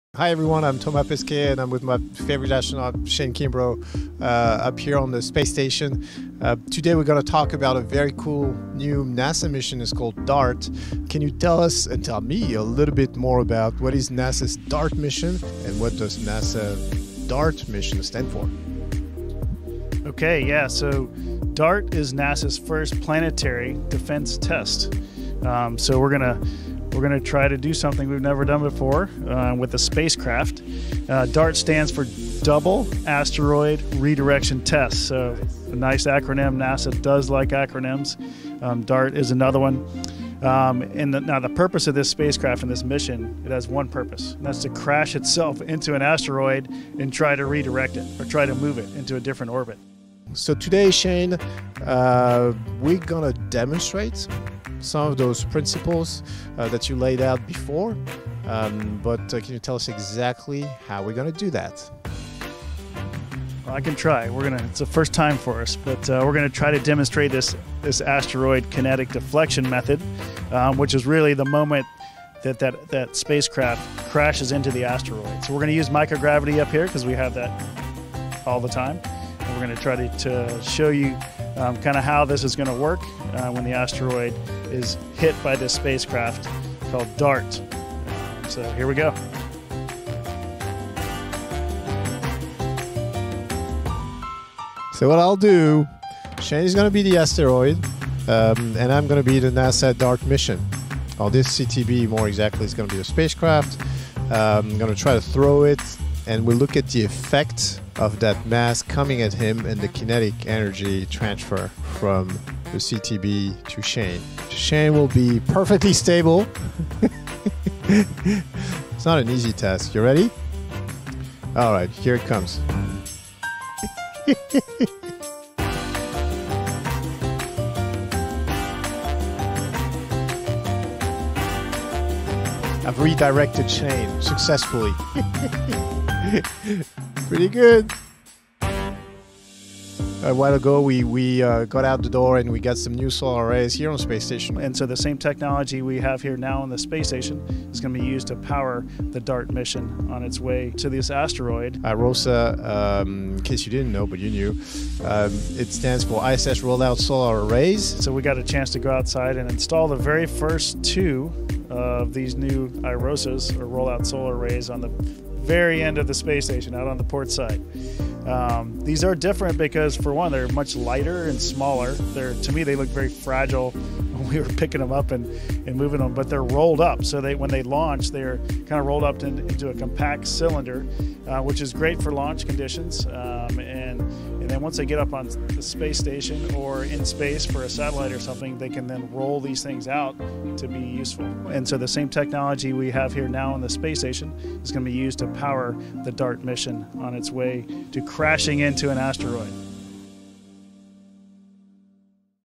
NASA DART Misyonu | Türkçe Altyazılı
NASA'nın yeni görevi DART hakkında paylaştığı videonun türkçe altyazılı videosu. KTÜ Uzay Kulübü üyeleri Seda Karaca ve Halil İbrahim Keleş'in türkçeye çevirdiği videoda DART misyonu hakkında NASA ve ESA astronotların ağzından dinliyoruz.

Credits: NASA